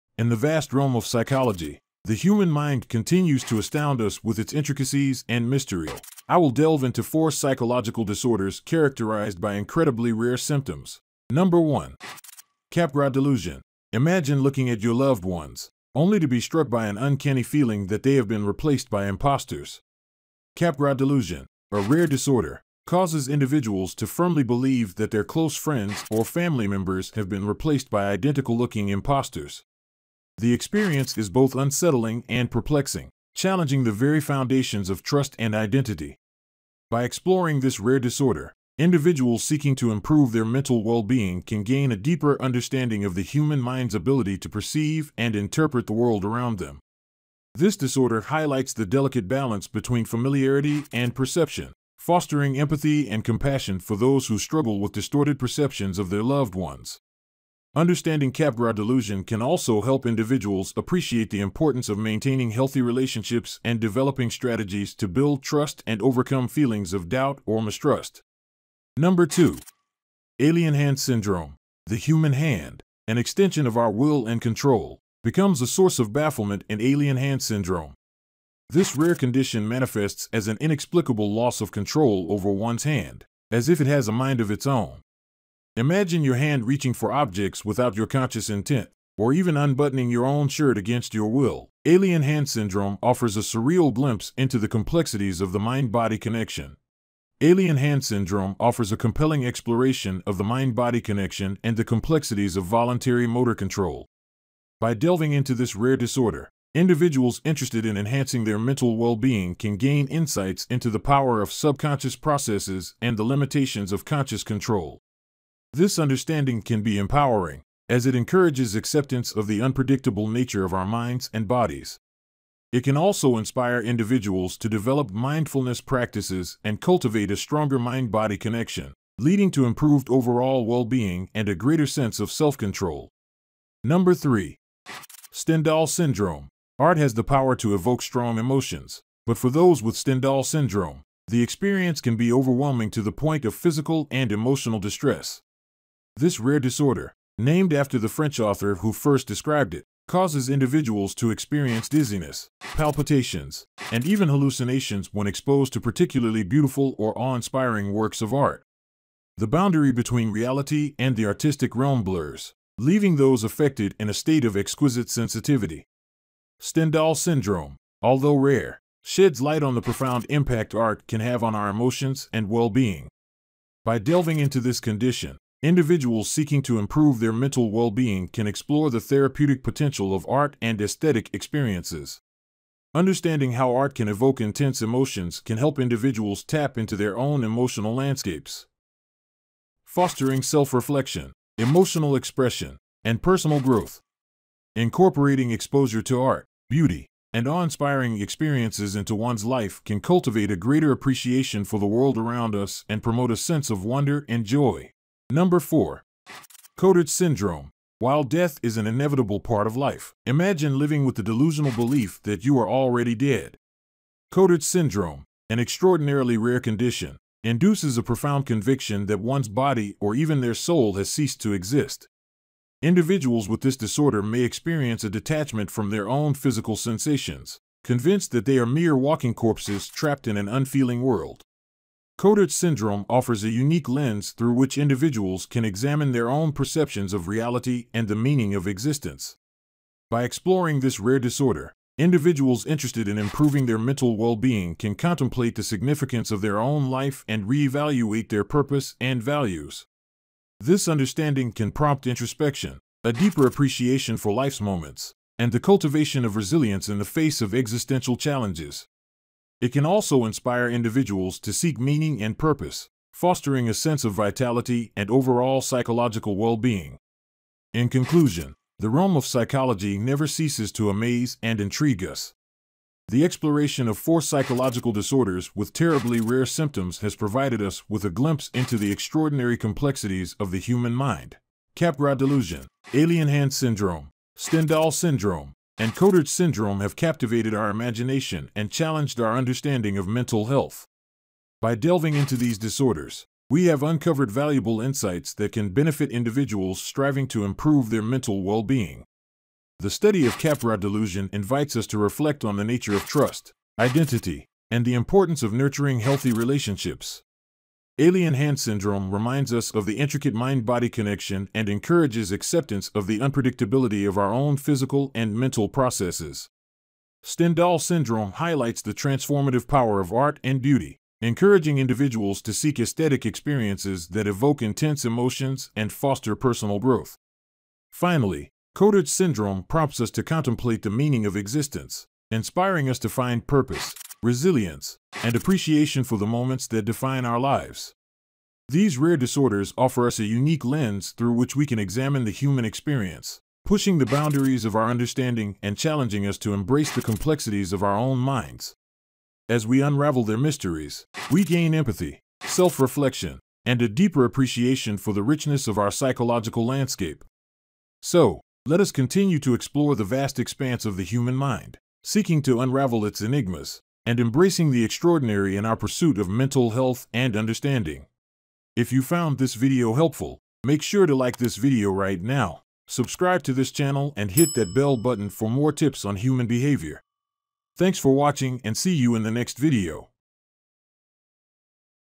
4 Psychological Disorder With Terrible Rare Symptoms
Are you fascinated by the complexities of the human mind? In this video, we delve into the intriguing world of psychological disorders and explore four rare disorders with terrible and unusual symptoms. From the rarest psychological disorders to strange and bizarre manifestations, we uncover the unique challenges faced by individuals with these conditions.

Join us as we navigate through the following disorders:

Bipolar Disorder: Discover the lesser-known symptoms and manifestations of bipolar disorder, an emotional rollercoaster that affects mood, energy levels, and daily functioning.

Borderline Personality Disorder: Explore the complex world of borderline personality disorder and its unique set of symptoms, including unstable relationships, intense emotions, and an unstable sense of self.

Schizotypal Personality Disorder: Uncover the strange and peculiar symptoms of schizotypal personality disorder, characterized by odd beliefs, eccentric behavior, and difficulties in forming close relationships.

Rare and Uncommon Psychological Disorders: Delve into the realm of rare psychological disorders that often go unnoticed, such as Cotard's Syndrome, Alien Hand Syndrome, or Capgras Delusion. These disorders exhibit bizarre and rare symptoms that challenge our understanding of the human mind.

Through this exploration of psychological disorders with terrible and rare symptoms, we aim to raise awareness, foster understanding, and encourage empathy towards individuals who experience these unique challenges. Join us on this journey to unravel the mysteries of the mind.